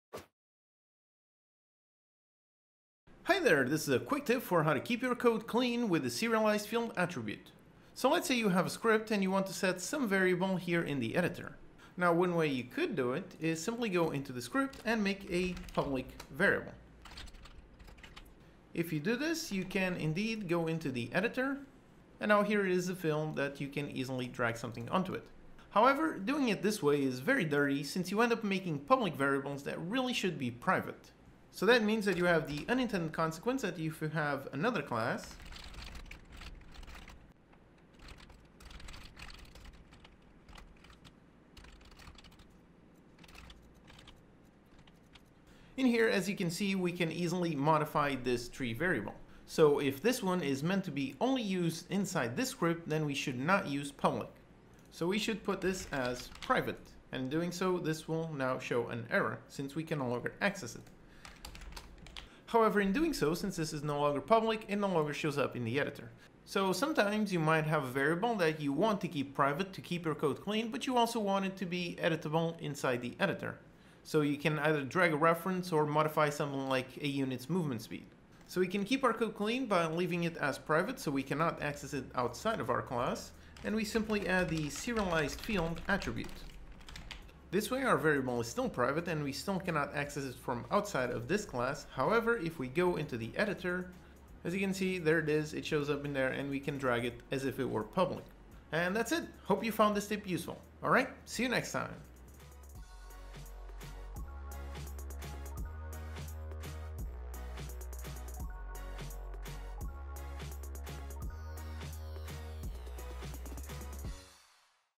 Quick Tip: Show Private fields in the Editor (Unity Tutorial)
Don't set variables public if they should be private. Use the [SerializeField] attribute to show private fields in the Editor.